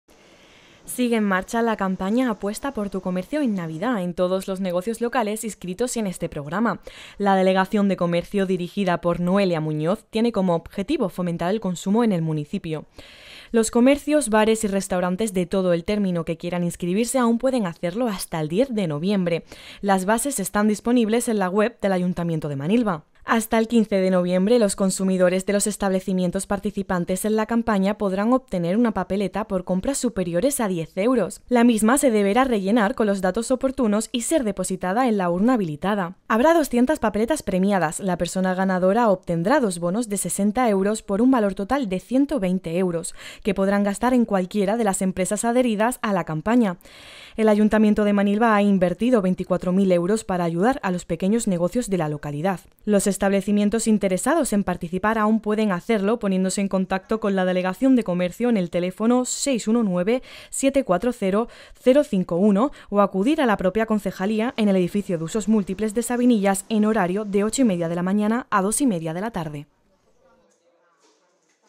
Continúa la campaña ‘Apuesta por tu comercio en Navidad’
Sigue en marcha la campaña ‘Apuesta por tu comercio en Navidad’ en todos los negocios locales inscritos en este programa. La delegación de Comercio, dirigida por Noelia Muñoz, tiene como objetivo fomentar el consumo en el municipio.

Los comercios, bares y restaurantes de todo el término que quieran inscribirse aún pueden hacerlo hasta el 10 de noviembre. Las bases están disponibles en la web del Ayuntamiento de Manilva.

Hasta el 15 de noviembre los consumidores de los establecimientos participantes en la campaña podrán obtener una papeleta por compras superiores a 10 euros. La misma se deberá rellenar con los datos oportunos y ser depositada en la urna habilitada.

Habrá 200 papeletas premiadas. La persona ganadora obtendrá 2 bonos de 60 €, por un valor total de 120 €, que podrán gastar en cualquiera de las empresas adheridas a la campaña.
El Ayuntamiento de Manilva ha invertido 24.000 euros para ayudar a los pequeños negocios de la localidad.

Los establecimientos interesados en participar aún pueden hacerlo poniéndose en contacto con la Delegación de Comercio en el teléfono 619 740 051 o acudir a la propia concejalía, en el Edificio de Usos Múltiples de Sabinillas, en horario de 08:30h a 14:30h.